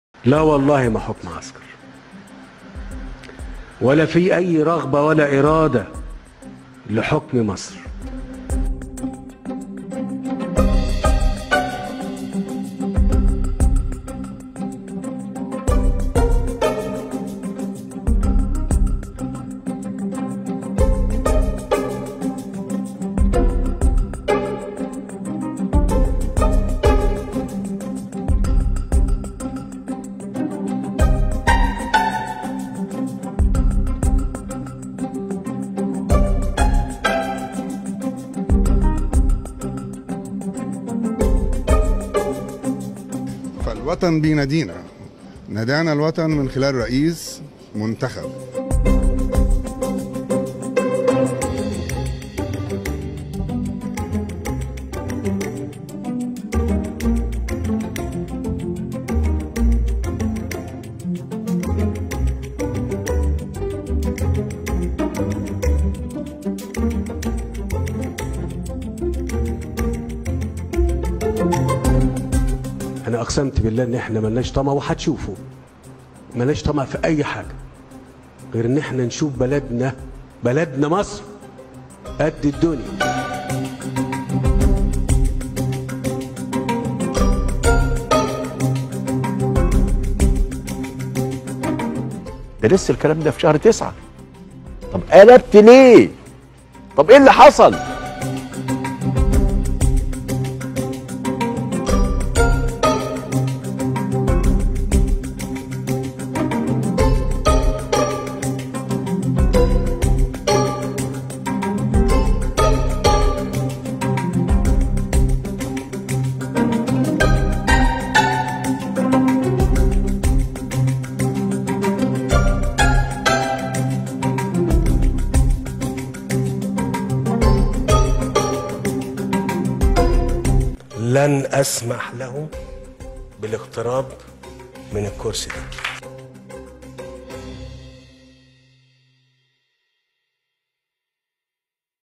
دعوة لترشيح صهر السيسي بديلا للسيسي!
"أصل مصر تركة أبوهم بيسلموها لبعض".. عماد جاد، المتحدث باسم التيار الحر (معارض) يفاجئ المصريين بدعوة صهر السيسي الفريق محمود حجازي رئيس أركان الجيش الأسبق إلى الترشح للرئاسة، أي تعويض السيسي بسيسي آخر!